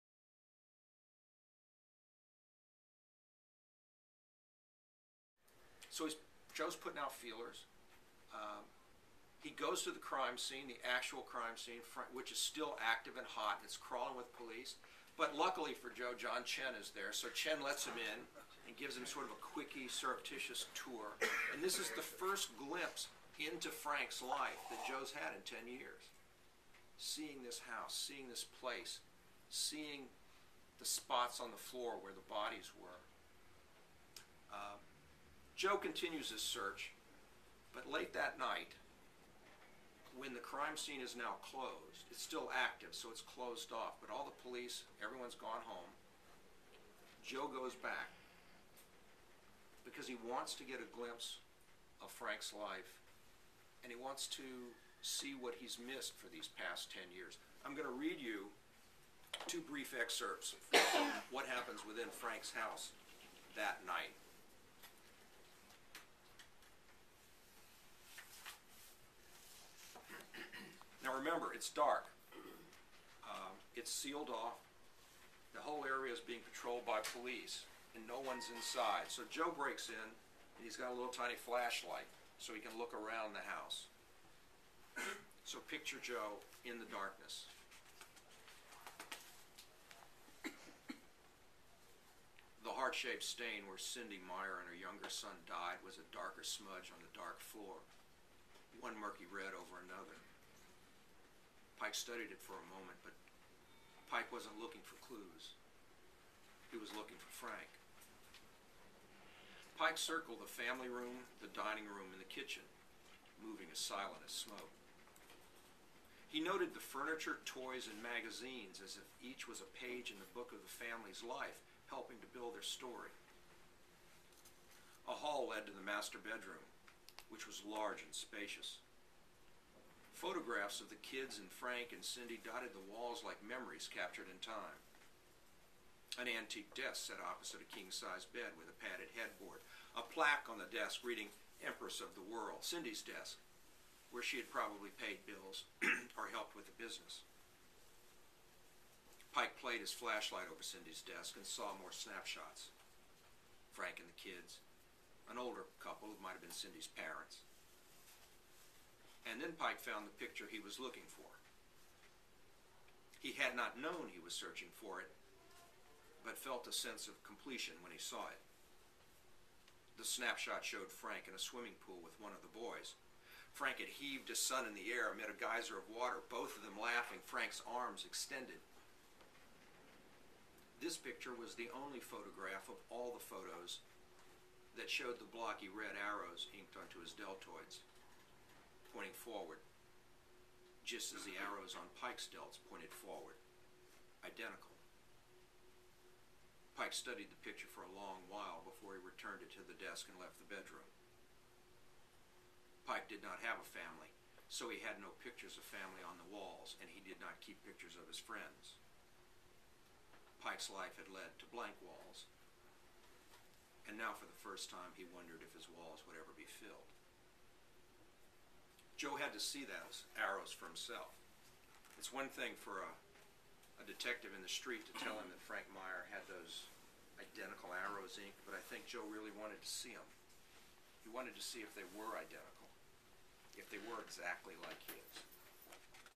Crais - First Rule - Reading 1
Author Robert Crais reads from his latest novel, THE FIRST RULE.  1 of 2.